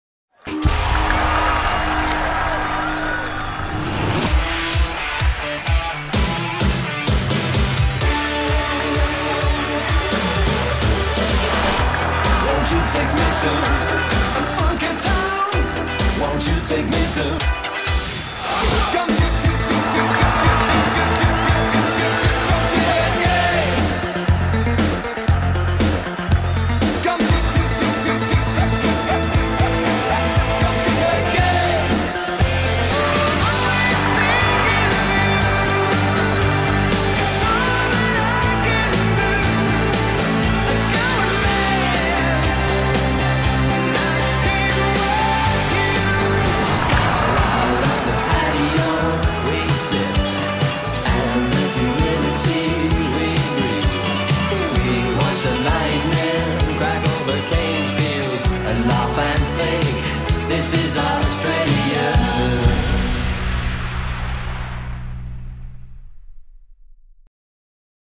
Pure Gold Live
Palais Theatre
1 Night, 12 Acts, 30 Massive Hits
Featuring: Ross Wilson - Dragon - John Paul Young - Wendy Matthews  - The Chantoozies - 1927  - Rose Tattoo  - Pseudo Echo - GANGgajang - Mi-Sex - Eurogliders - Chocolate Starfish